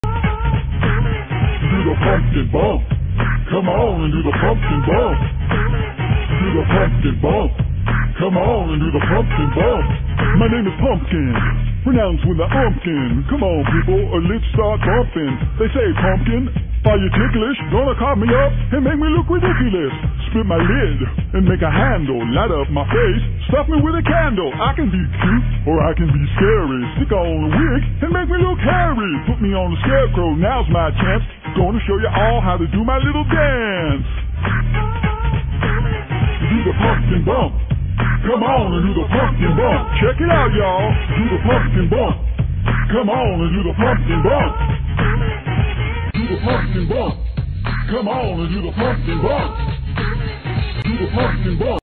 "Seedy Pumpkin Bumpin' " - FlowGo animation
"Seedy Pumpkin Bumpin' " is an animation created by FlowGo in 2002/2003, now archived for Youtube.